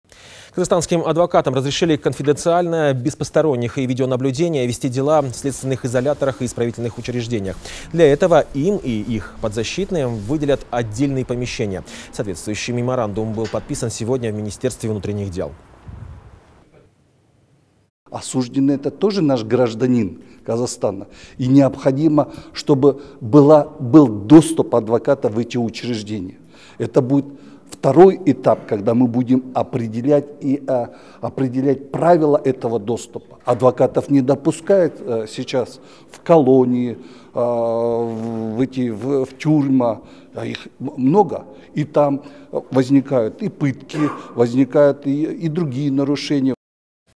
Адвокатам разрешили конфиденциально оказывать правовую помощь в СИЗО
Казахстанским адвокатам разрешили конфиденциально, без посторонних и видеонаблюдения вести дела в следственных изоляторах и исправительных учреждениях.

Для этого им и их подзащитным выделят отдельные помещения. Соответствующий меморандум был подписан сегодня в министерстве внутренних дел.

Ануар Тугель, председатель республиканской коллегии адвокатов

- Осужденный – это тоже наш гражданин, необходимо, чтобы был доступ адвоката в эти учреждения. Это будет второй этап, когда мы будем определять правила этого доступа. Адвокатов сейчас не допускают в колонии, тюрьмы. Там возникают и пытки и другие нарушения.